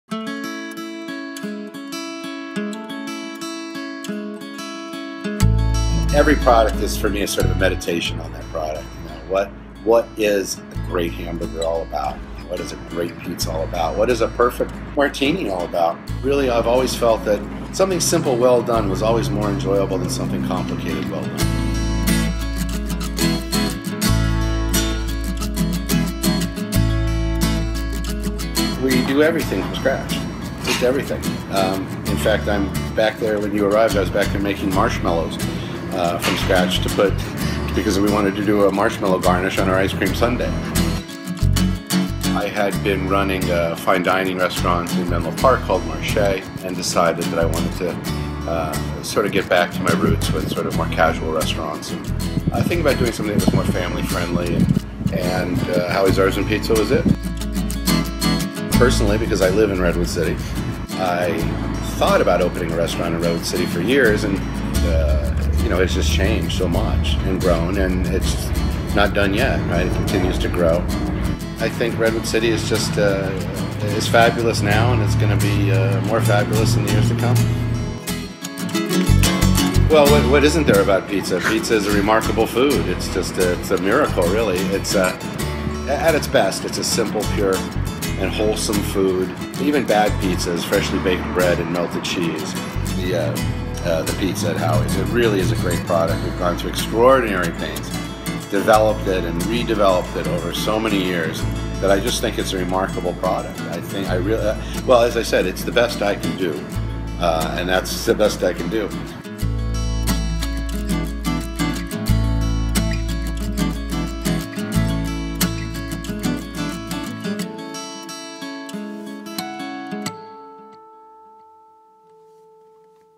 Voices of Downtown - Howie's Pizza
In this video interview for the City of Redwood City, Howie of Howie's Artisan Pizza tells us how his restaurants make everything from scratch (even marshmallows!), and how he left the world of fine dining for the pure, down-to-earth joy of pizza. He thinks of every dish as a "meditation" on that dish.